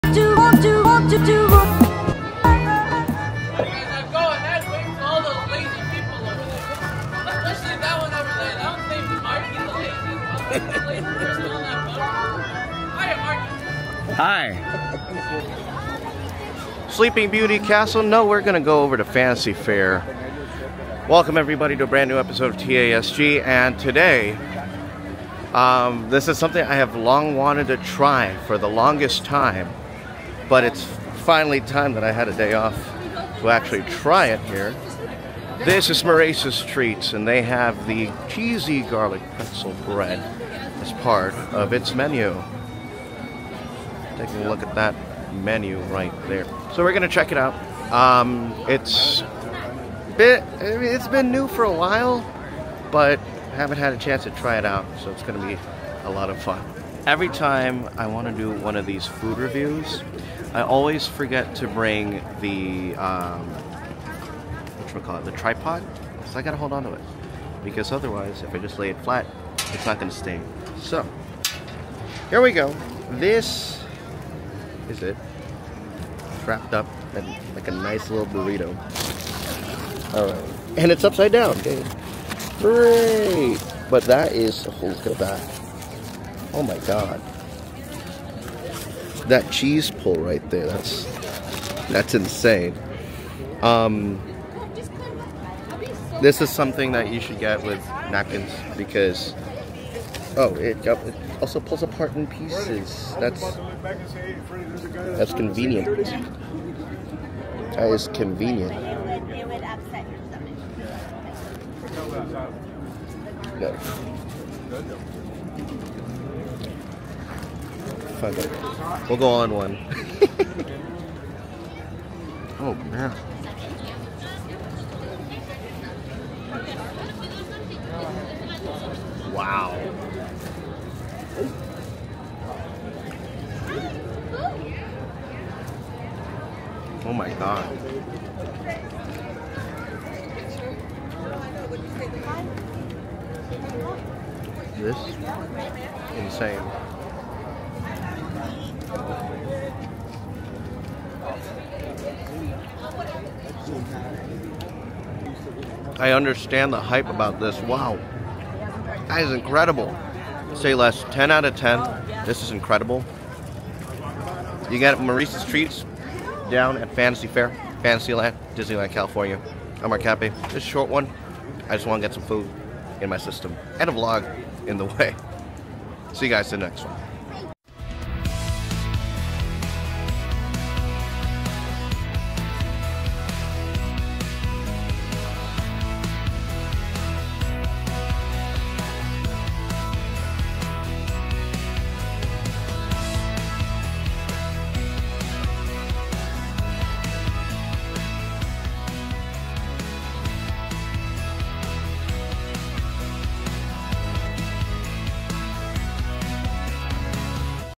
MY NEW FAVORITE FOOD ITEM
Checking out the hottest food item out of Maurice’s Treats at Disneyland Park.